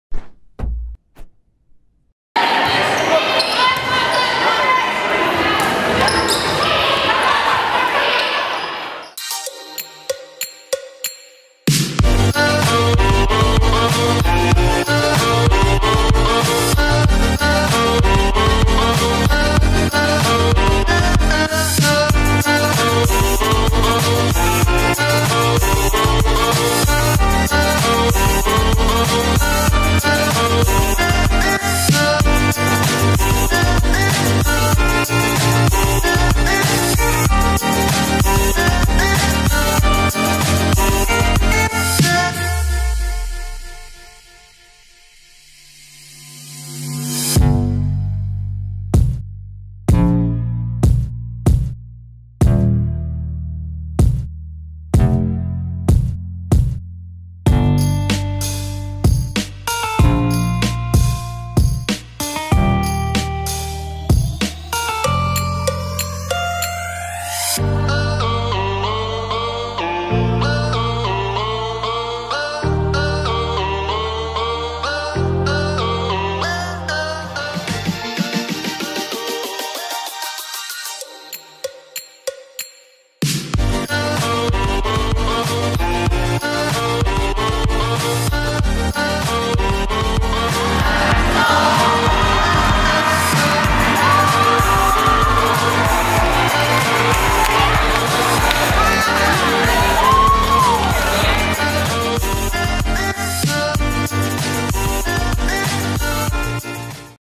Texas Tech vs UTEP
The Texas Tech Women's Volleyball team beat UTEP 3-0 September 5th in the United Supermarkets Arena practice gym.